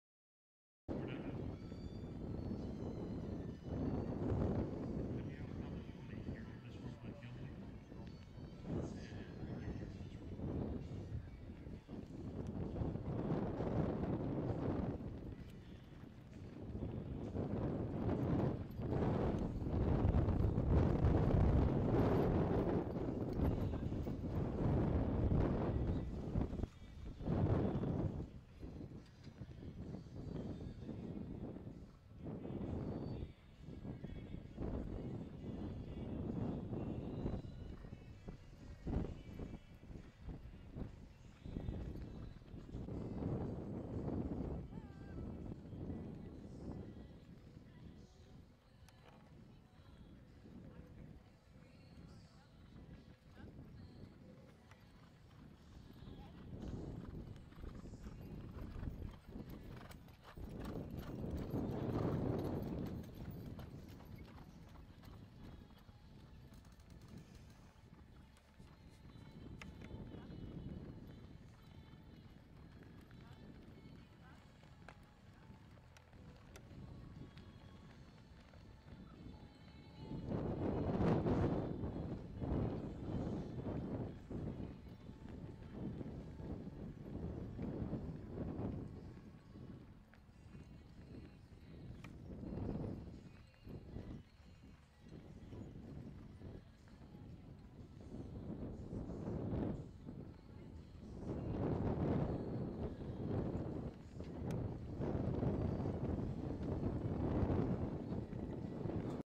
Misdee Wrigley Miller Dressage LER 2012
This test driven at Little Everglades CDE in Feb  2012 was Misdee's best test of the Florida season, scoring 43.73